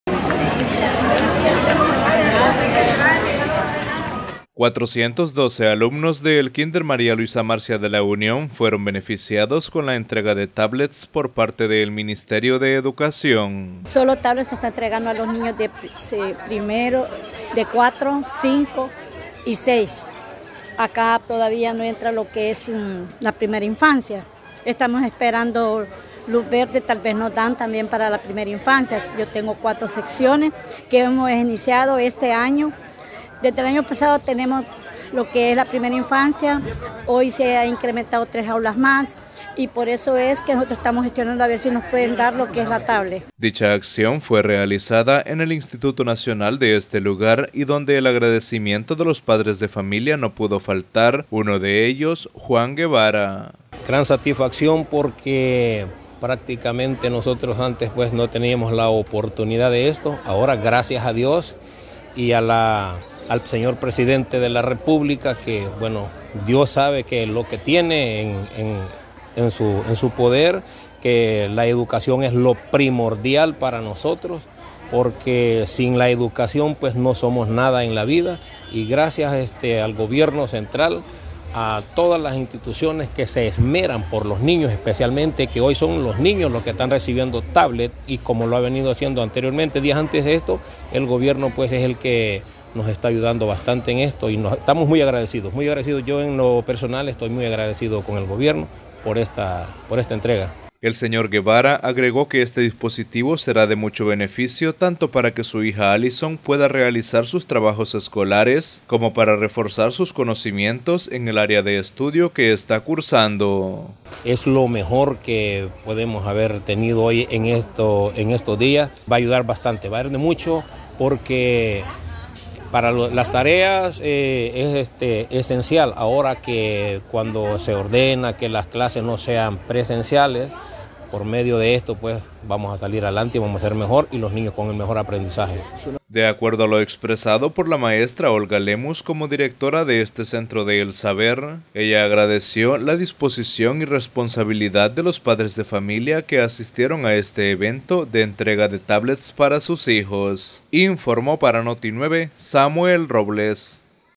ENTREGA DE TABLETS
En la ciudad de La Unión, más de 400 niños del kínder María Luisa Marcia, fueron beneficiados con la entrega de tabletas por parte del Ministerio de Educación en el marco del programa Enlace con la Educación, de esta manera el gobierno del presidente Bukele sigue cerrando la brecha digital en el país.